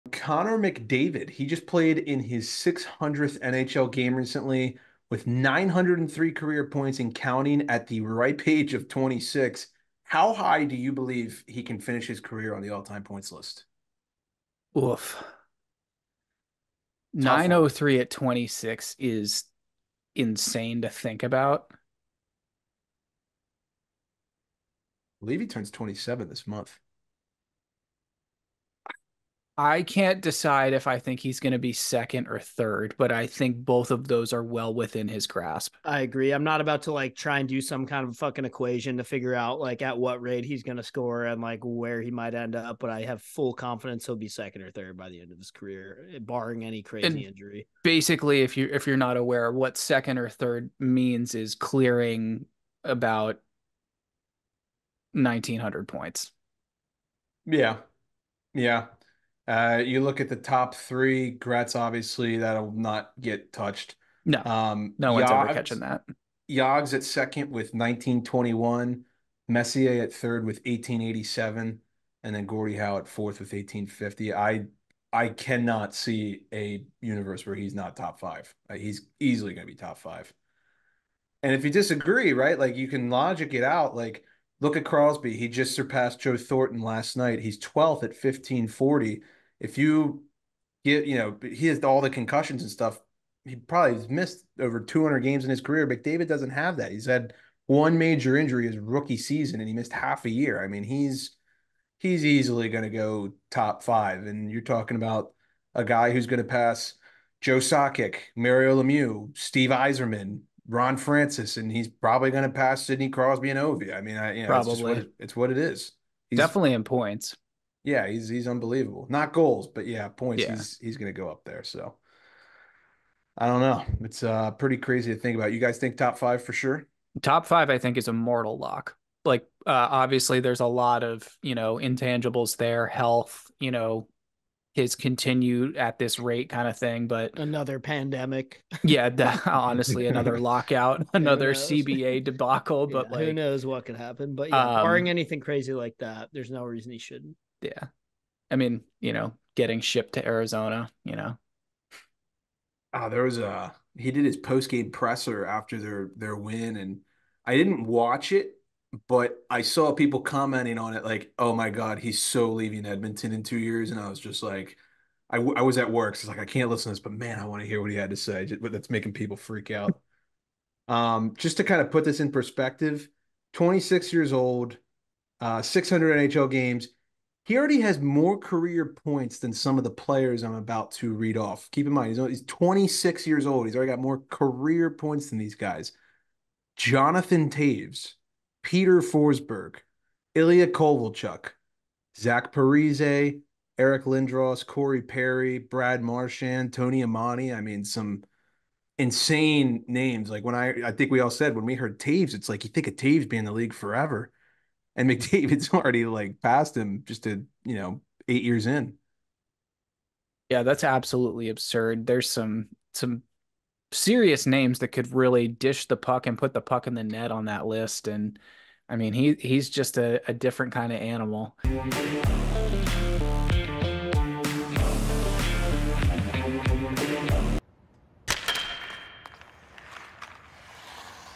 Where will McDavid end up on the ALL TIME points list??
Thumbnail : Perry Nelson, USA Today Sports